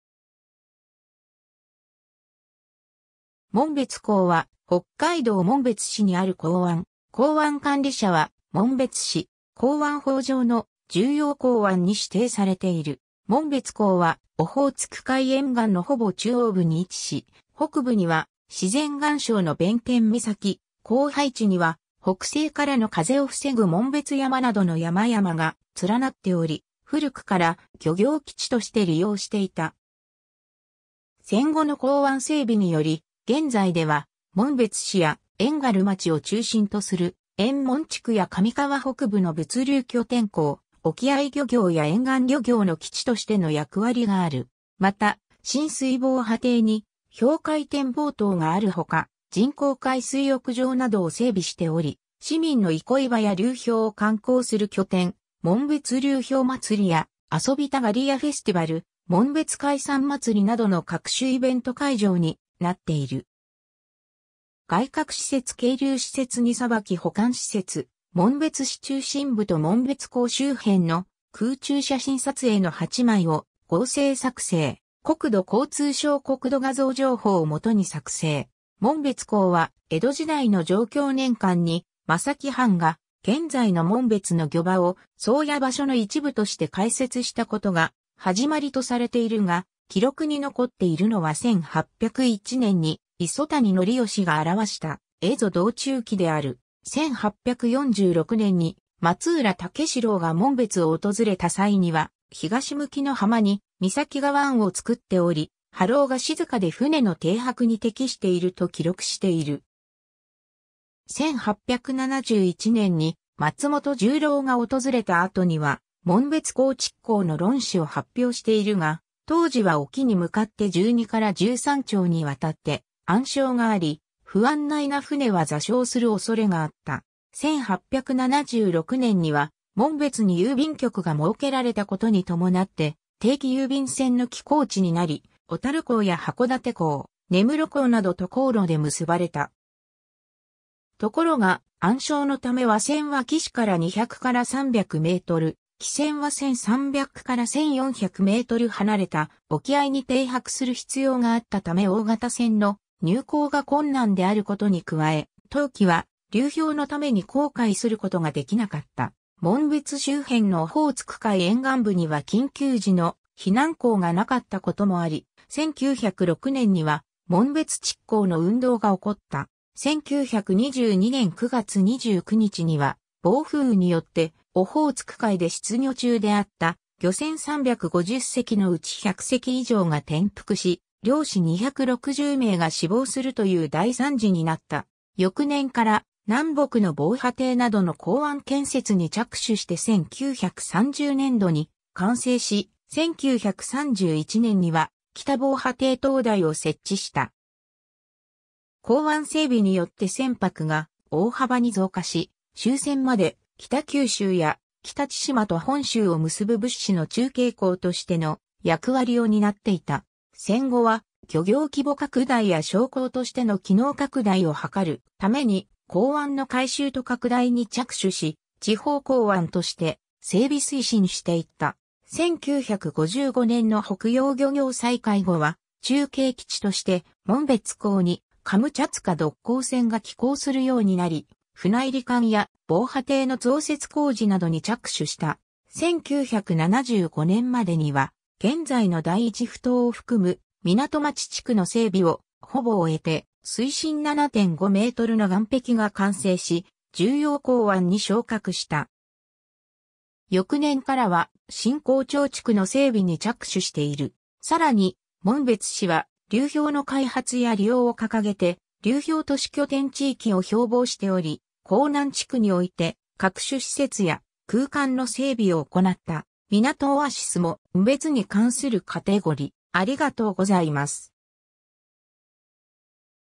紋別港
紋別港（もんべつこう）は、北海道紋別市にある港湾。
港湾管理者は紋別市。
港湾法上の「重要港湾」に指定されている。
 紋別港はオホーツク海沿岸のほぼ中央部に位置し、北部には自然岩礁の弁天岬、後背地には北西からの風を防ぐ紋別山（大山）などの山々が連なっており、古くから漁業基地として利用していた。
戦後の港湾整備により、現在では紋別市や遠軽町を中心とする遠紋地区や上川北部の物流拠点港、沖合漁業や沿岸漁業の基地としての役割がある。
また、親水防波堤（クリオネプロムナード）に氷海展望塔（オホーツクタワー）があるほか、人工海水浴場（オホーツクホワイトビーチ）などを整備しており、市民の憩い場や流氷を観光する拠点、『もんべつ流氷まつり』や『遊びたガリヤフェスティバル』、『もんべつ海産まつり』などの各種イベント会場になっている。
 外郭施設 係留施設 荷さばき保管施設 紋別市中心部と紋別港周辺の空中写真（1978年（昭和53年）撮影の8枚を合成作成）。
 紋別港は、江戸時代の貞享年間に松前藩が現在の紋別の漁場を「ソウヤ場所」の一部として開設したことが始まりとされているが、記録に残っているのは1801年（享和元年）に磯谷則吉が著した『蝦夷道中記』である。
1846年（弘化3年）に松浦武四郎が紋別を訪れた際には、東向きの浜に岬が湾を作っており、波浪が静かで船の停泊に適していると記録している。
1871年（明治4年）に松本十郎が訪れた後には紋別港築港の論旨を発表しているが、当時は沖に向かって12〜13丁（約1,300〜1,400 m）に渡って暗礁があり、不案内な船は座礁する恐れがあった。
1876年（明治9年）には紋別に郵便局が設けられたことに伴って定期郵便船の寄港地になり、小樽港や函館港、根室港などと航路で結ばれた。
ところが、暗礁のため和船は岸から200〜300 m、汽船は1,300〜1,400 m離れた沖合いに停泊する必要があったため大型船の入港が困難であることに加え、冬季は流氷のために航海することができなかった。
紋別周辺のオホーツク海沿岸部には緊急時の避難港がなかった事もあり、1906年（明治39年）には紋別築港の運動が起こった。
1922年（大正11年）9月29日には暴風雨によってオホーツク海で出漁中であった漁船350隻のうち100隻以上が転覆し、漁師260名が死亡するという大惨事になった。
翌年から南北の防波堤などの港湾建設に着手して1930年度（昭和5年度）に完成し、1931年（昭和6年）には北防波堤灯台を設置した。
港湾整備によって船舶が大幅に増加し、終戦まで北九州や北千島と本州を結ぶ物資の中継港としての役割を担っていた。
 戦後は漁業規模拡大や商港としての機能拡大を図るために港湾の改修と拡大に着手し、「地方港湾」として整備推進していった。
1955年（昭和30年）の北洋漁業再開後は、中継基地として紋別港にカムチャツカ独航船（仲積船）が寄港するようになり、船入澗や防波堤の増設工事などに着手した。
1975年（昭和50年）までには現在の第1ふ頭を含む港町地区の整備をほぼ終えて水深7.5 mの岸壁が完成し、「重要港湾」に昇格した。
翌年からは新港町地区（現在の第2ふ頭）の整備に着手している。
さらに、紋別市は流氷の開発や利用を掲げて「流氷都市拠点地域」を標榜しており、港南地区において各種施設や空間の整備を行った。
 みなとオアシスもんべつに関するカテゴリ: